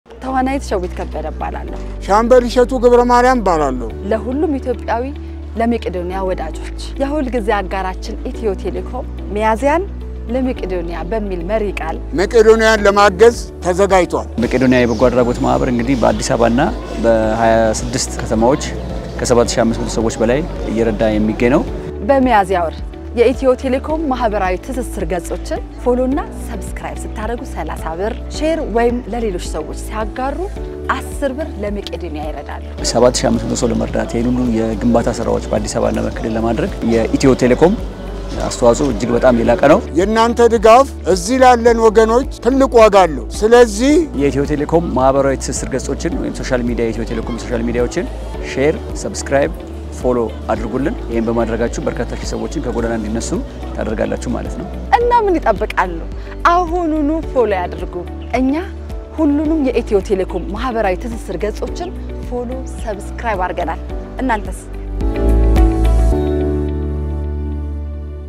ሚያዝያን ለመቄዶንያ!
በዚህ ወር በማኅበራዊ ትስስር ገፆቻችን በሚቀላቀሉ አዲስ ተከታዮች (Followers) ብዛት ልክ 30 ብር እንዲሁም በሚጋሩ ልጥፎች (Share) ልክ 10 ብር ለመቄዶንያ አረጋዊያን እና ህሙማን መርጃ ማዕከል እንለግሳለን!

እርስዎም ይፋዊ የማኅበራዊ ትስስር ገፆቻችንን ይከተሉ፤ ፖስቶቻችንን ያጋሩ፤ ለወገን ድጋፍ ይተባበሩ!

ዘላቂ የጋራ ማኅበራዊ ኃላፊነት ለወገን ብሩህ ተስፋ!